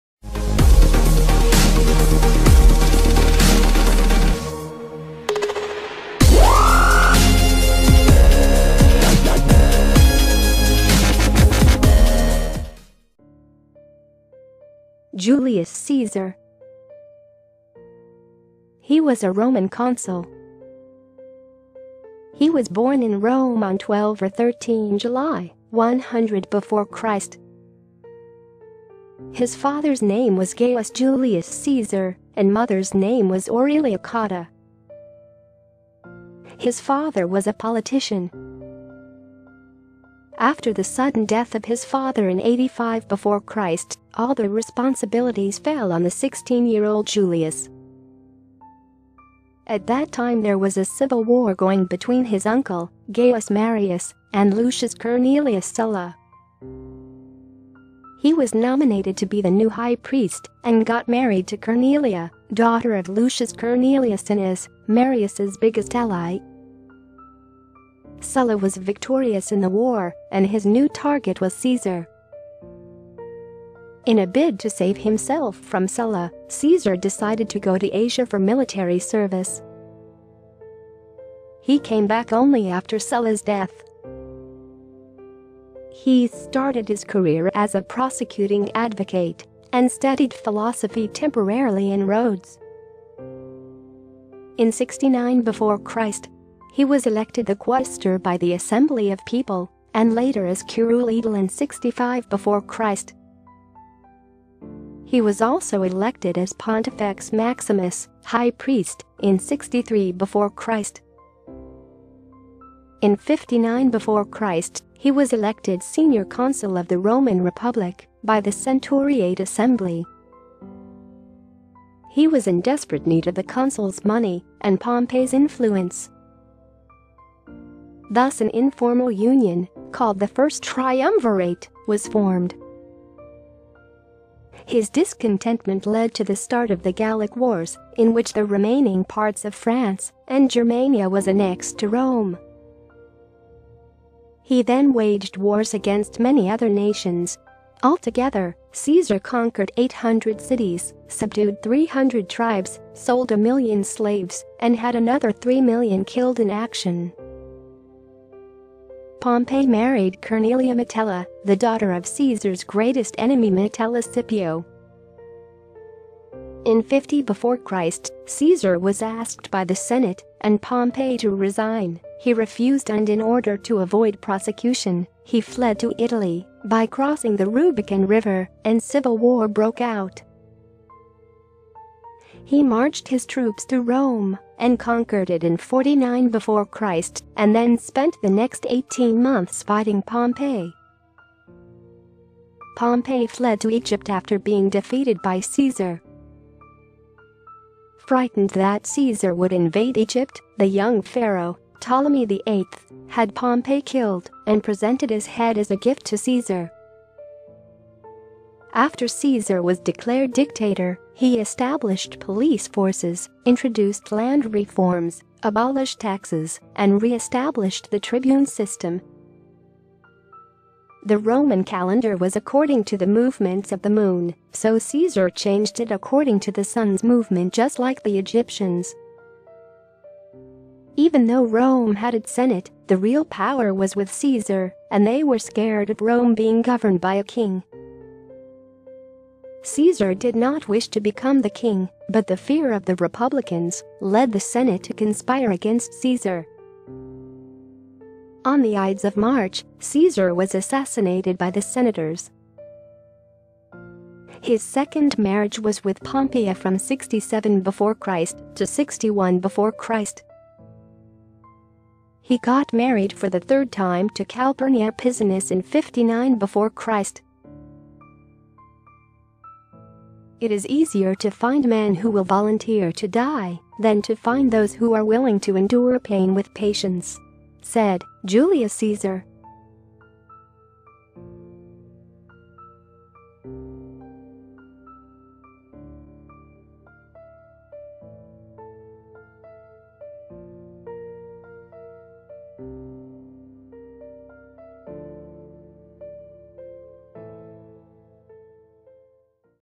Julius Caesar Short Biography - Former Roman Consul
Here in this video I have described about Julius Caesar Short Biography - Former Roman Consul. I hope you will really like this Julius Caesar Short Biography - Former Roman Consul video. Gaius Julius Caesar, known by his nomen and cognomen Julius Caesar, was a Roman politician, military general, and historian who played a critical role in the events that led to the demise of the Roman Republic and the rise of the Roman Empire. He is also known as an author of Latin prose.